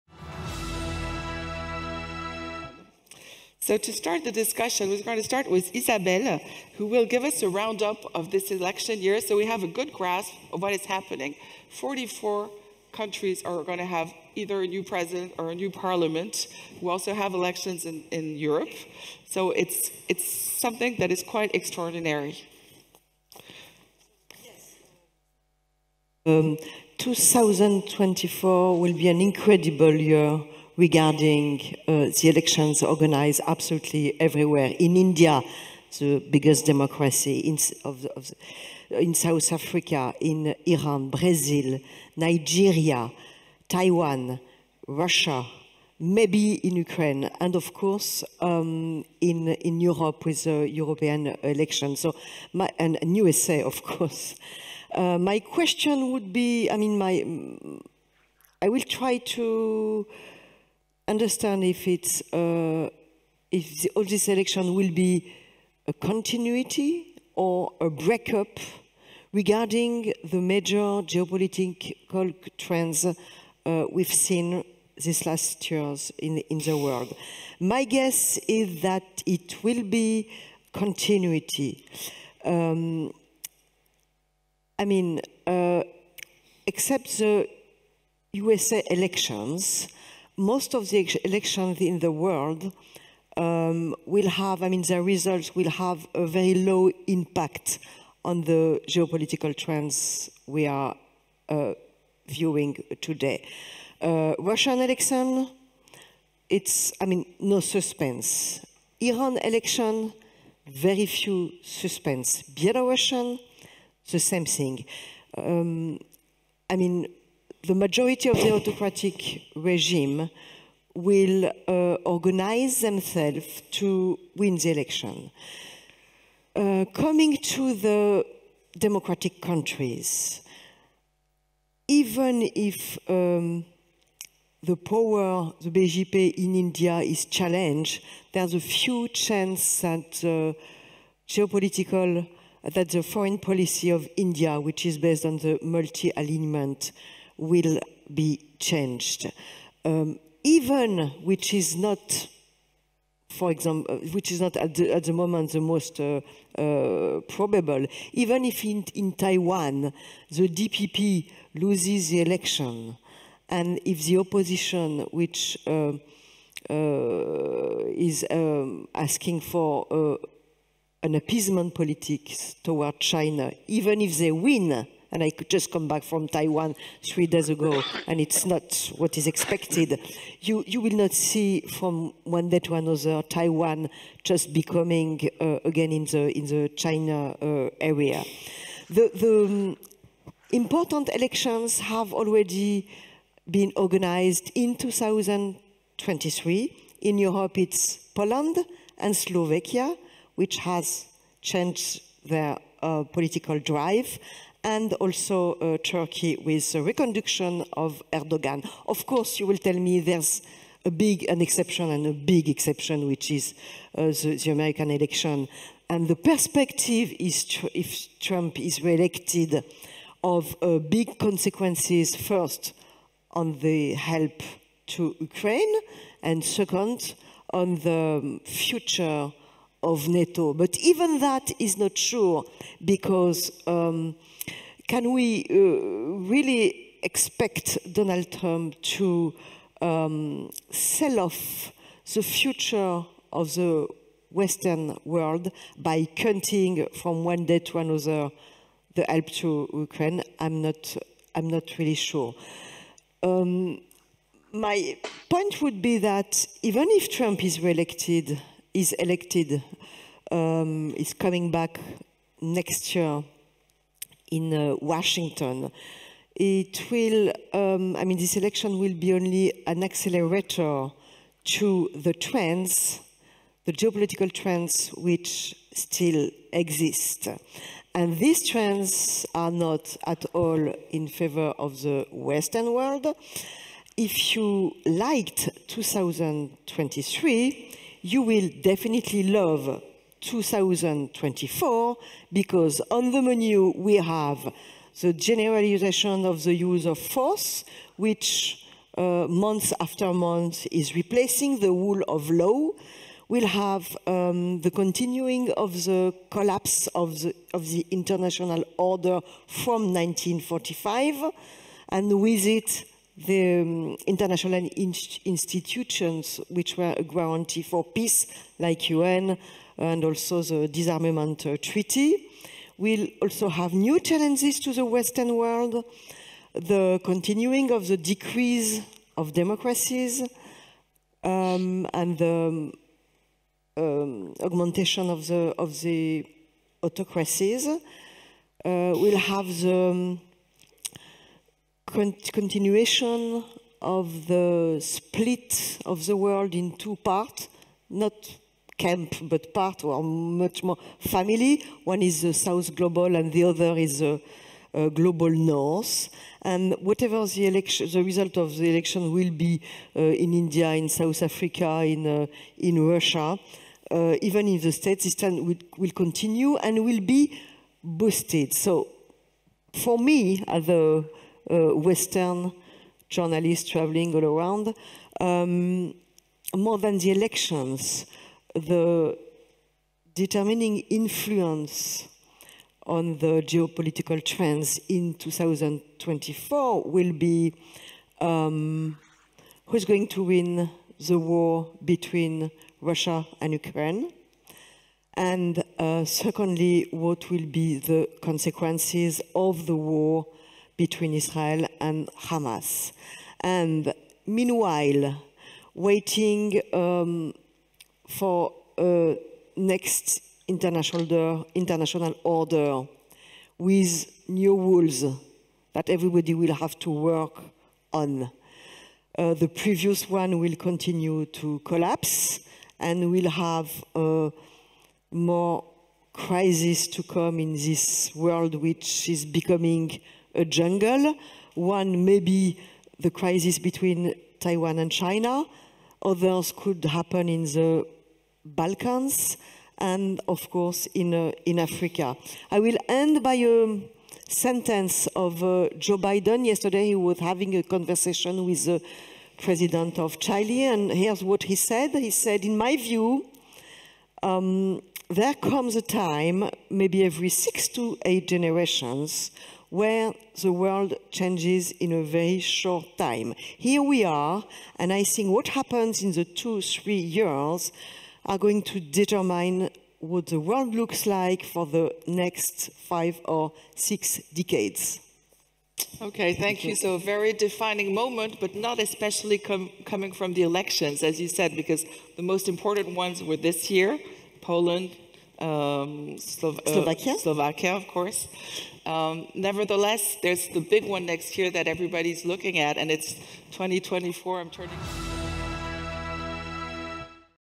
WPC 2023 - Isabelle Lasserre - Plenary session 8: 2024: A Critical Electoral Year
Isabelle Lasserre, Diplomatic Correspondent for Le Figaro